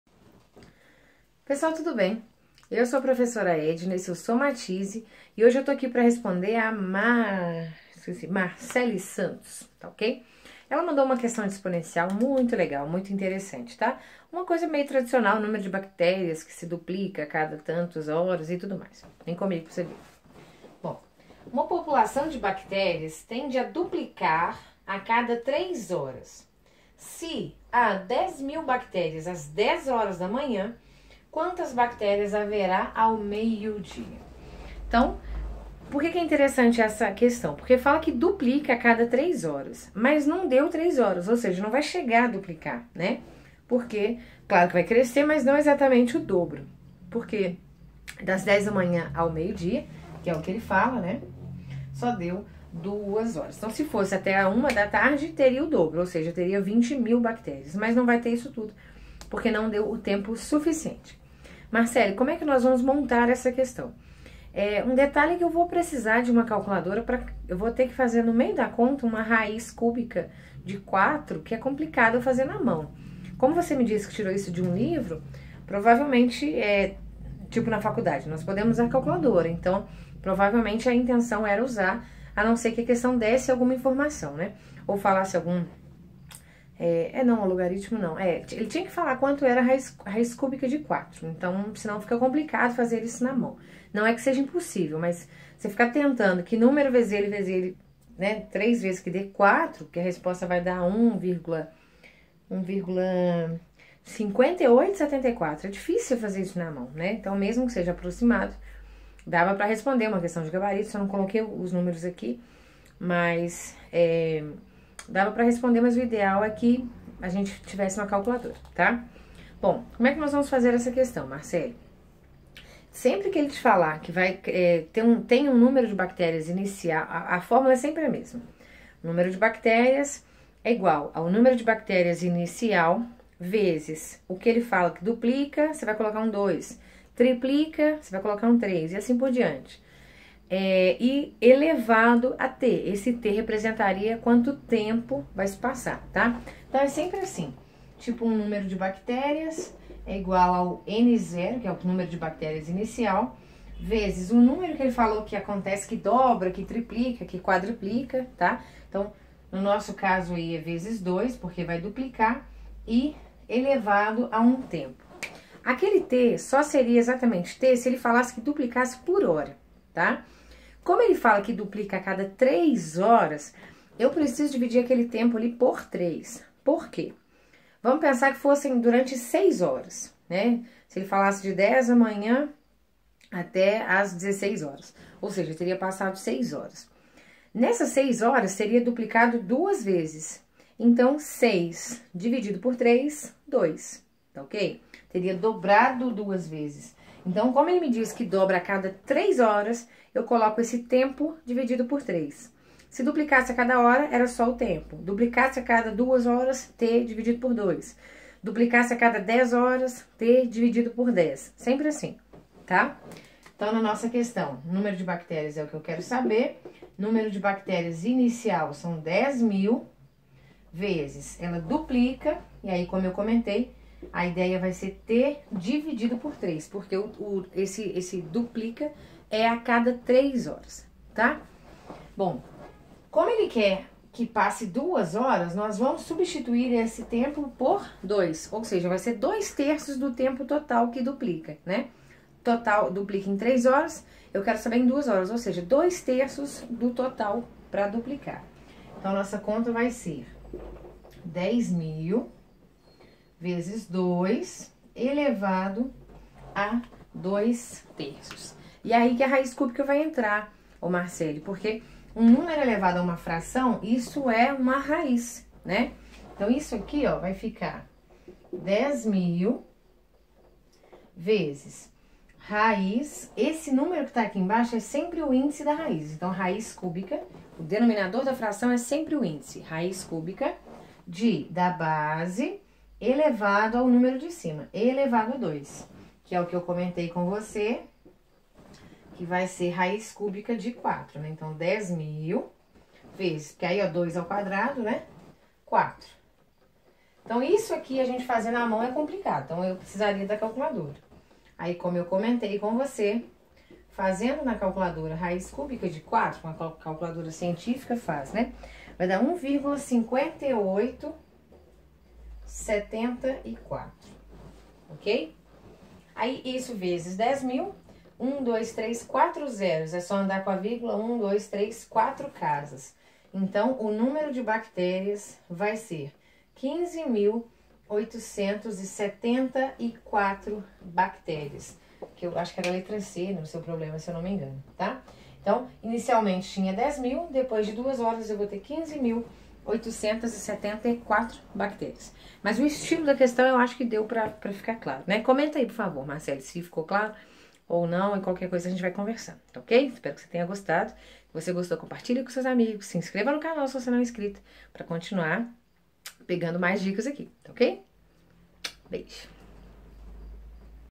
Exercício de função exponencial - Nº de bactérias dobra a cada 3 horas - Prof Edna
Nos siga no instagram: @somatize_edna